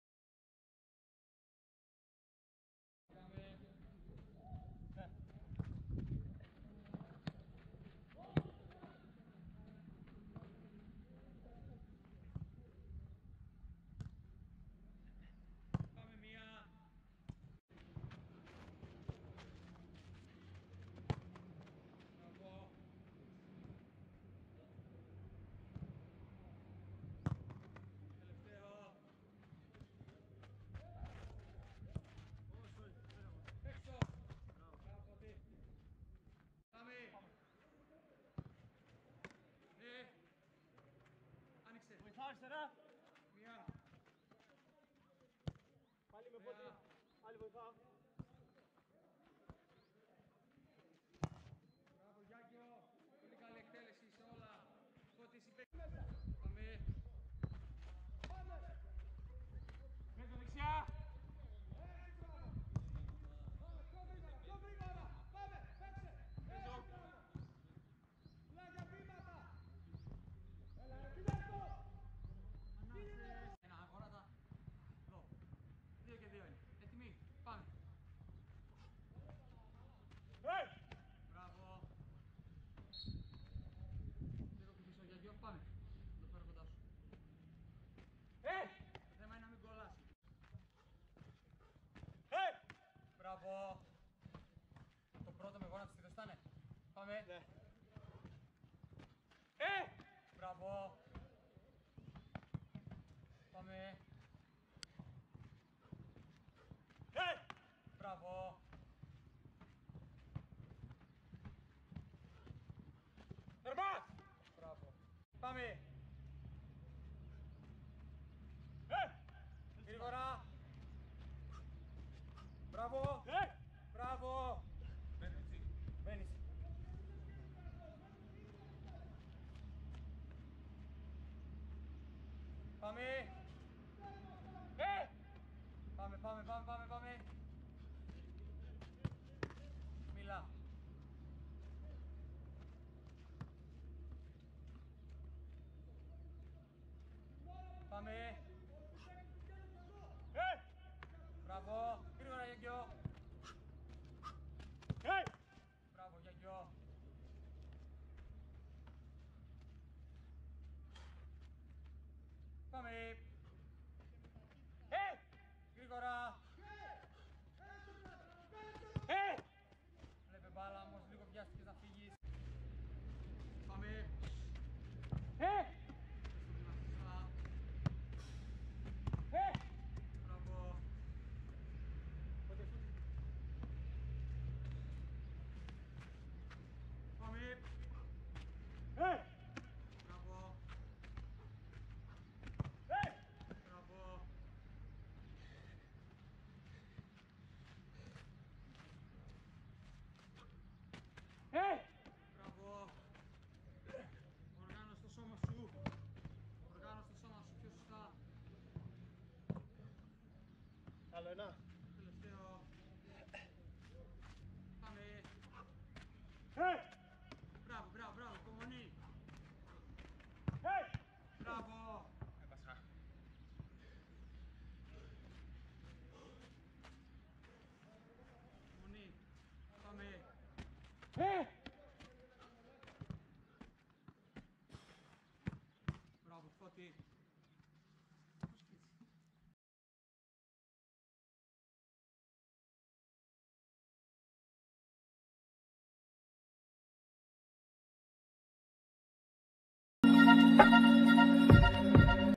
Goalkeeper Training Philosophy new v1 "Distribution & High Contour Catch"
Main aims of the Training session:

Technical - One-touch and two-touch distribution / Handling / Side Contour Catch / High Contour Catch

Tactical - Communication / Supporting Angle / Positioning

Physical - Plyometric Training / Anaerobic Power

Mental - Decision Making

Note: In this particular session communication is very important. The GK needs to Communicate with his team during the build-up of their attack. Communication is also very important for the second part of the session which is about Side Contour Catching and High Contour Catching. Don't forget to 1) Call early, 2) Stay calm, 3) Be specific and 4) Encourage your teammates.

GKS: Giagkiozis Giorgos, Sgouris Fotis